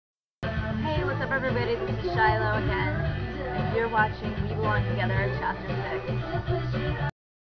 We Belong Together Chapter 6
The girls began to take off their shirts and shorts and revealed their bikinis to the boys. Kylie had on a bright blue bikini, Kiera had a bright purple bikini on, and Shiloh had a bright green tankini on, feeling self-conscious about her weight.

Kevin: Awe, crap...

Joe: What?

Nick: *Noticing the same thing Kevin did* We forgot our bathing suits...

Joe: Guess we'll have to swim nude...

Kevin and Nick: WHAT?

Joe: *giggling* Just kidding.

Kiera: Guys, come on!

Kylie: CATCH US IF YOU CAN!!!

The girls started running towards the water.

Kevin: Alright. Nick, you get Kylie, and Joseph, you take Kiera. Shiloh's all mine. GO!

The boys raced towards the girls who were already at water's edge.

Shiloh: EEEK! The water's so cold!!!

Kiera: You'll get used to it!!! *Dives in*

Kylie: Yeah! Do you really want to get caught?!?!? *dives in*

Shiloh: Hmm...well, that depends...are we being competitive or flirty?

Shiloh then heard Kevin running up behind her and dove into the water.

WHAT WILL HAPPEN? WILL THE BOYS CATCH THE GIRLS? DO THE GUYS HAVE CRUSHES ON THE GIRLS? DO THE GIRLS HAVE CRUSHES ON THE GUYS??? FIND OUT IN CHAPTER 7!!!